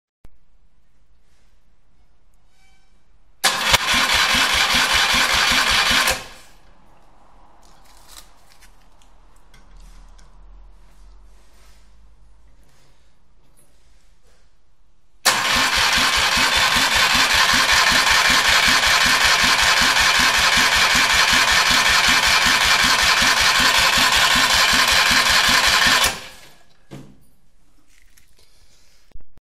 Daf 600
Motor geplaatst in de auto.

Eerste test van de start motor.